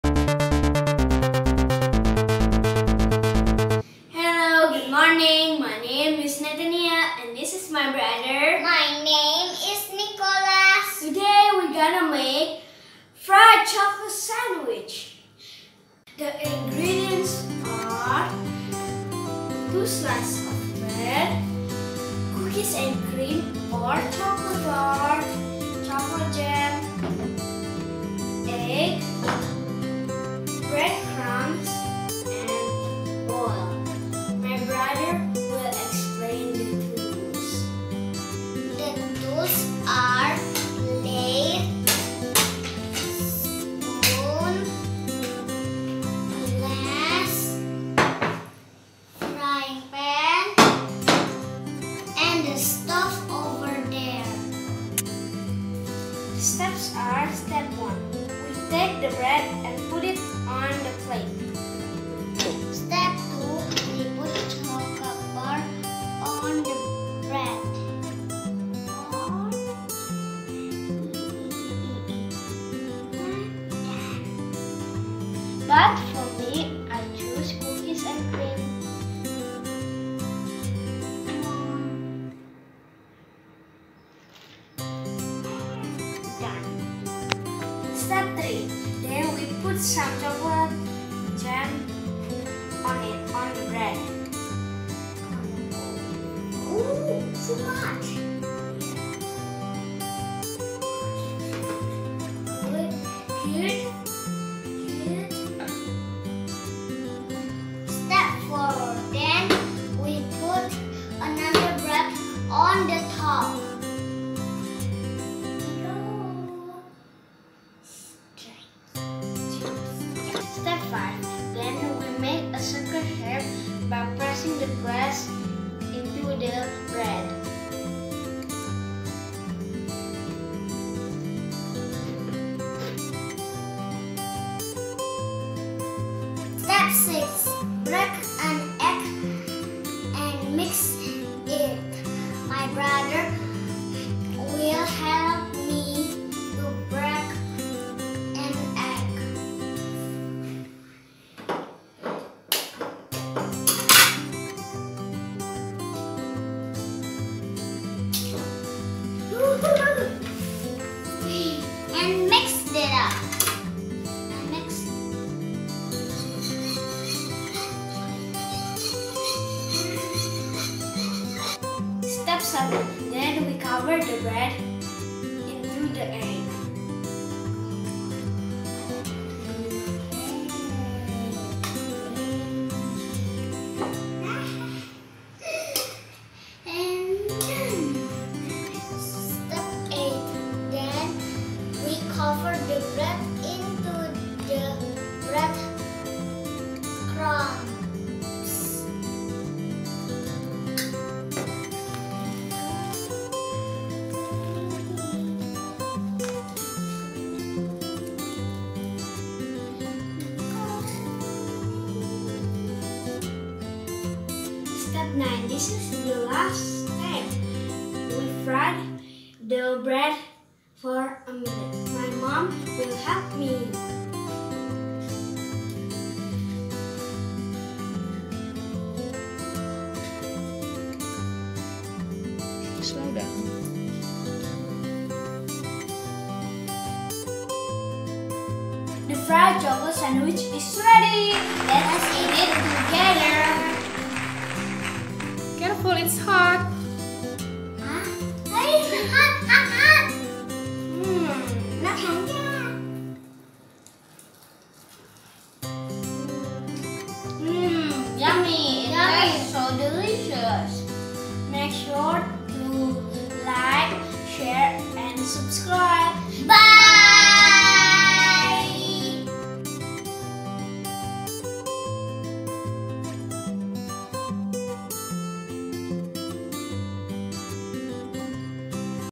Fun cooking class from home. FRIED CHOCOLATE SANDWICH
Haii Guys,

Kali ini NatNick mau fun cooking class untuk tugas tempat kursus Little Sunshine di singaraja, Bali.

Karena masih pandemi, jadinya kami kerjakan fun cooking class kami dari rumah.

Hari ini kita akan buat Fried Chocolate Sandwich. Yukk lihat keseruannya. Teman² juga bisa buat di rumah loh, ini enak dan cara buatnya mudah sekali 👍